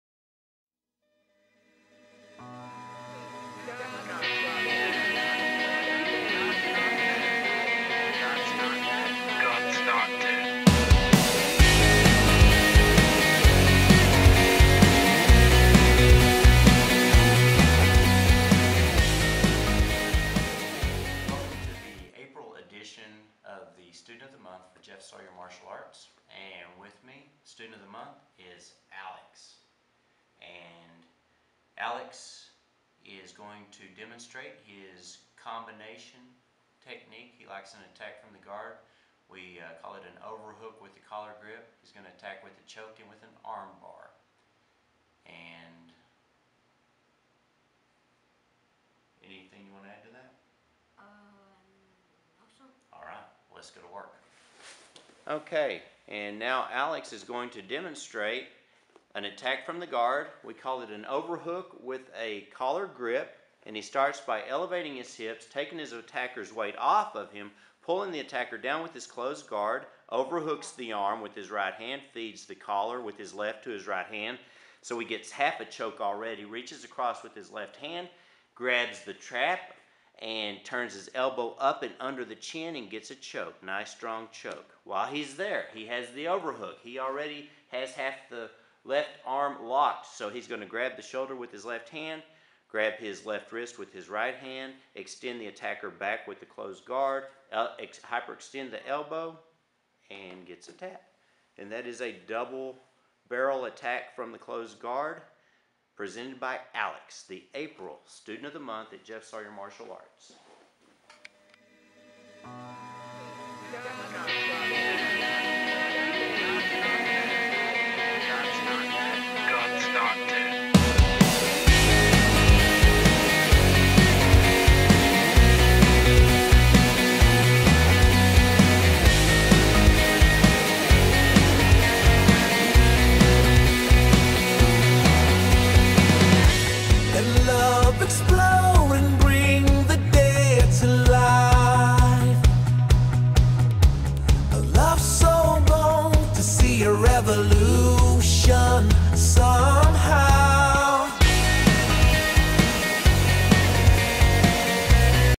HPBJJ April 2014 Student of the Month
Brazilian jiu jitsu technique video. Submission techniques from the guard.